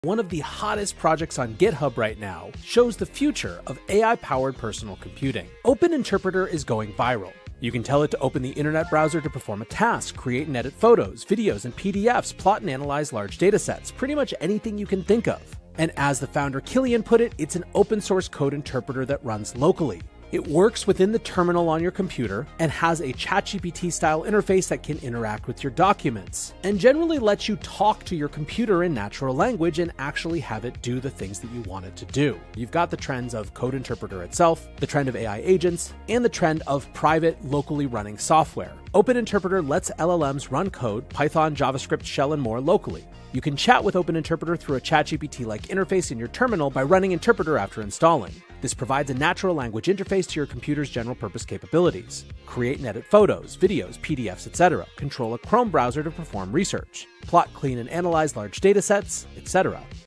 🔮 Is Open Interpreter the future of AI-powered computing? 💥
🔥 Don't sleep on this AI revolution! Open Interpreter is reshaping the landscape of AI-powered personal computing. Imagine commanding your computer in natural language and watch it perform tasks effortlessly. Dive into the future of AI agents and private, locally-run software. Open Interpreter is the key to running code, analyzing data, and more, all on your terms. 💡✨